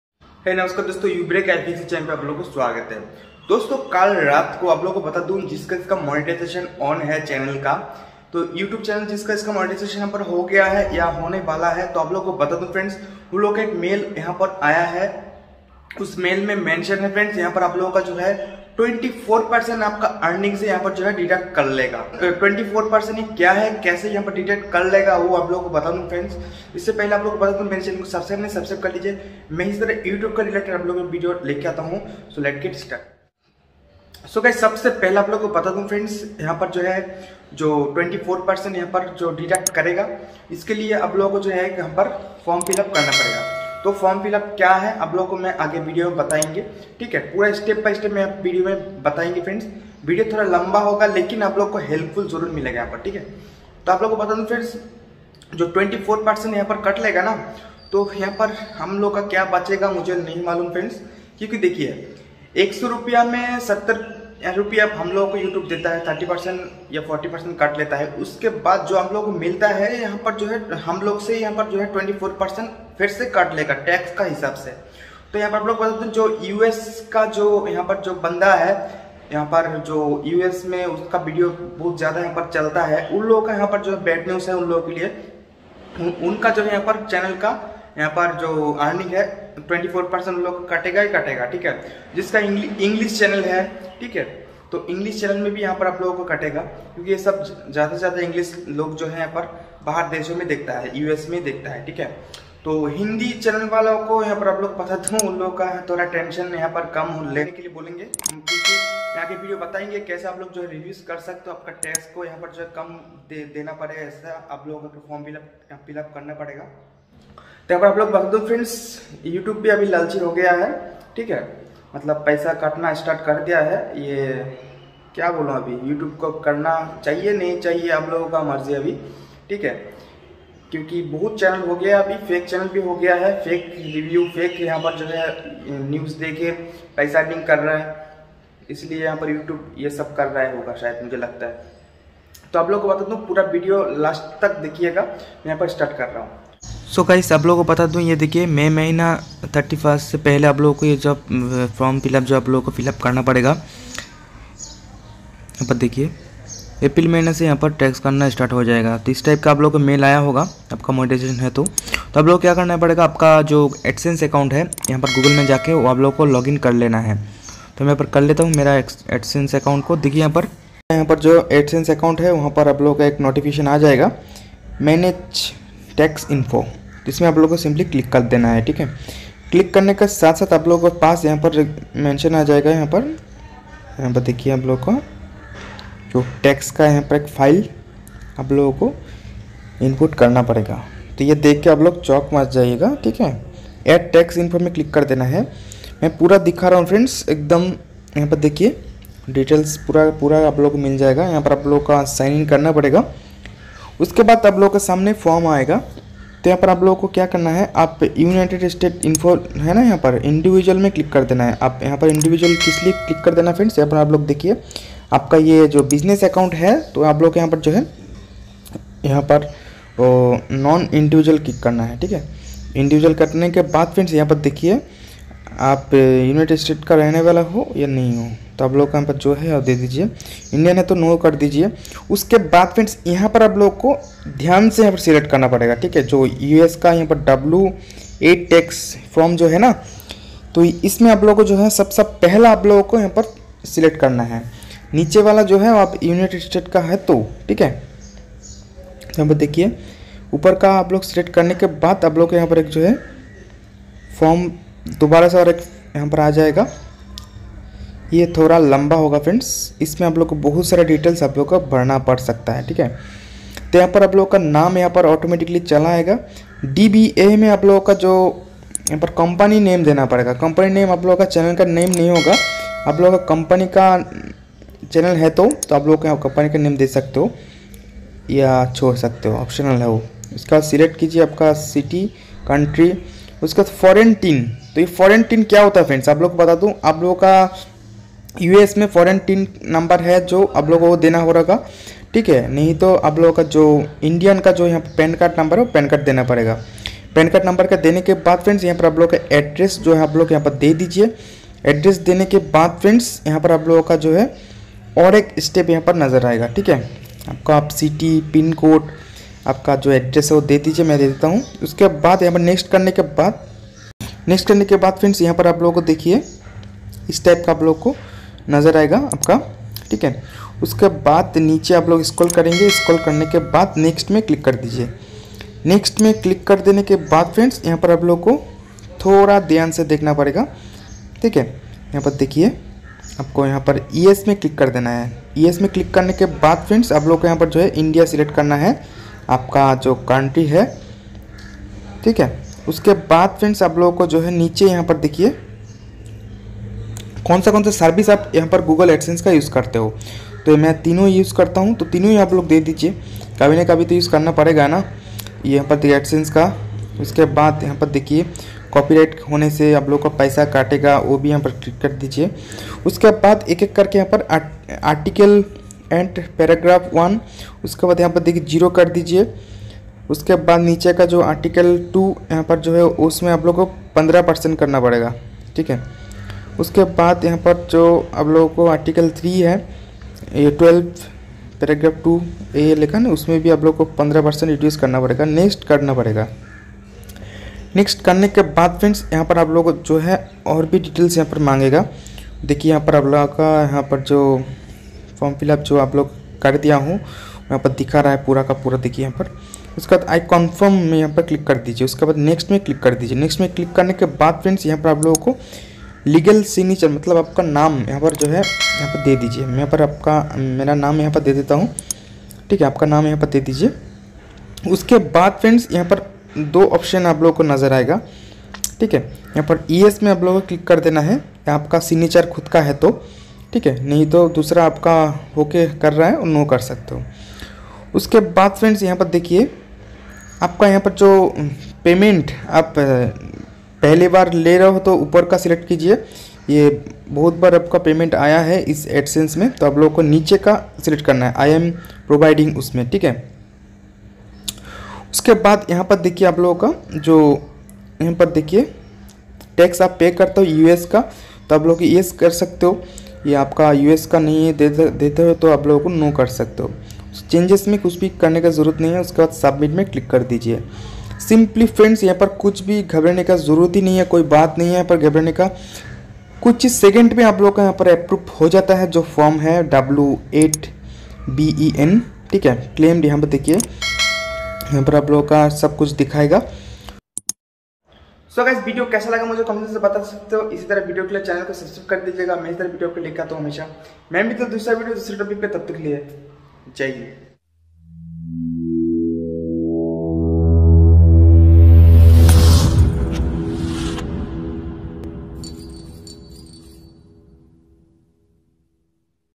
How to File TAX information W-8BEN TAX FORM in Google AdSense
How to File TAX information W-8BEN TAX FORM in Google AdSense | Youtube Blog & Admob 2021 FOR INDIA

All Monetizing creators on YouTube, regardless of their location in the world, are required to provide tax info. Please submit your tax info as soon as possible. If your tax info isn’t provided by May 31, 2021, Google may be required to deduct up to 24% of your total earnings worldwide.